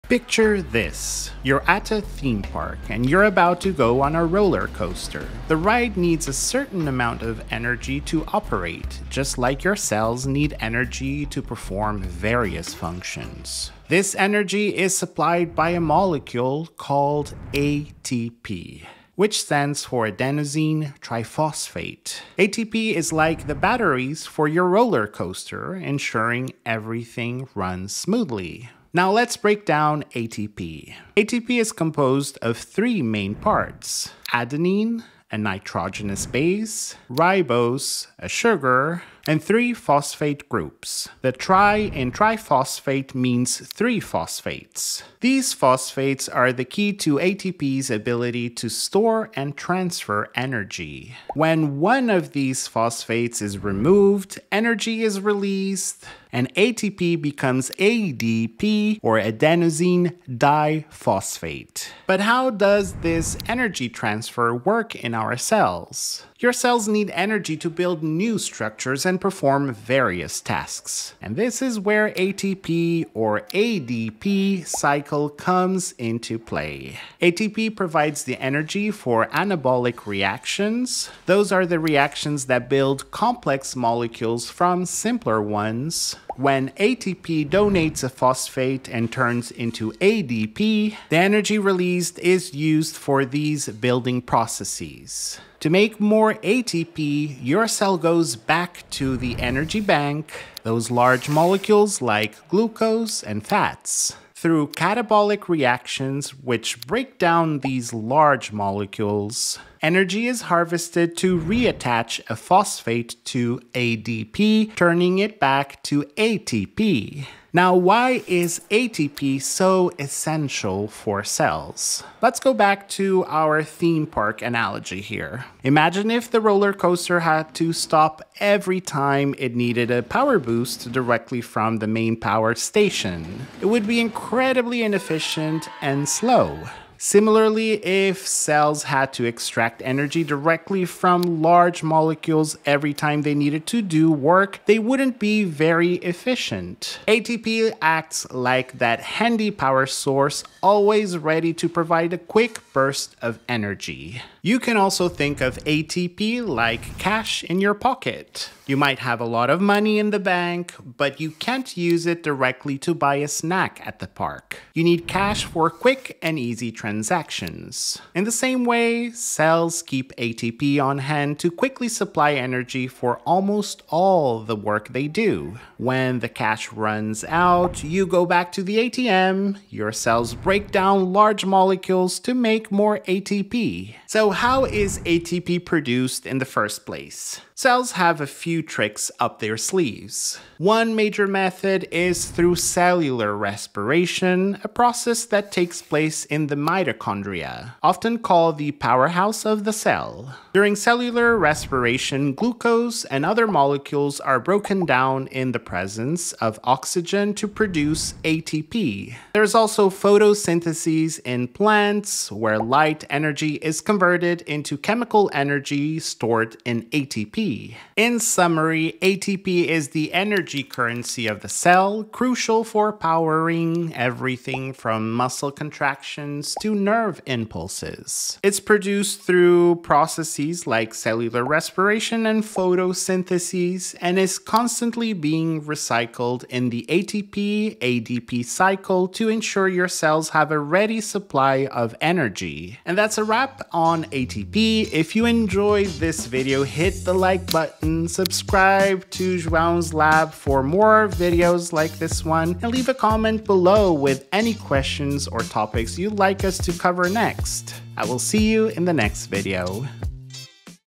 What is ATP (adenosine triphosphate)?
What is ATP? Imagine ATP as the energy currency of your cells, powering everything from muscle movement to nerve impulses! In this video, we break down adenosine triphosphate (ATP), how it stores and transfers energy, and why it’s essential for life. Learn about the ATP-ADP cycle, cellular respiration, and more in this easy-to-understand explanation.

And check out the great anatomy and physiology content we produce at Kenhub

Video chapters:
00:00 INTRO
00:28 ATP structure
01:01 Energy transfer
01:56 ATP functions
03:03 How is ATP produced?